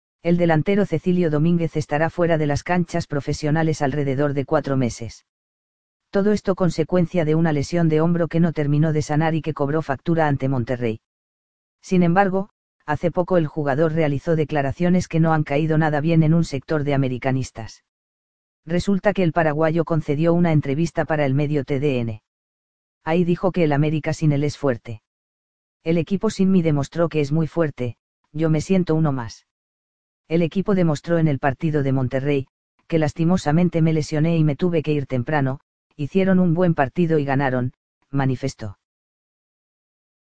Las declaraciones de Domínguez que han causado controversia!
El delantero Cecilio Domínguez estará fuera de las canchas profesionales alrededor de 4 meses. Todo esto consecuencia de una lesión de hombro que no terminó de sanar y que cobró factura ante Monterrey. Sin embargo, hace poco el jugador realizó declaraciones que no han caído nada bien en un sector de americanistas.

Resulta que el paraguayo concedió una entrevista para el medio TDN. Ahí dijo que el América sin él es fuerte:

"El equipo sin mí demostró que es muy fuerte, yo me siento uno más. El equipo demostró en el partido de Monterrey, que lastimosamente me lesioné y me tuve que ir temprano, hicieron un buen partido y ganaron", manifestó.